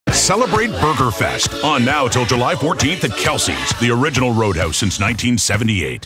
Kelsey's Famous Burger Fest Is Back!
Try our NEW burgers, like our mouth-watering Bacon Bourbon BBQ Burger or The Amazeballs Burger, made with 100% Canadian beef and stacked with exciting new toppings!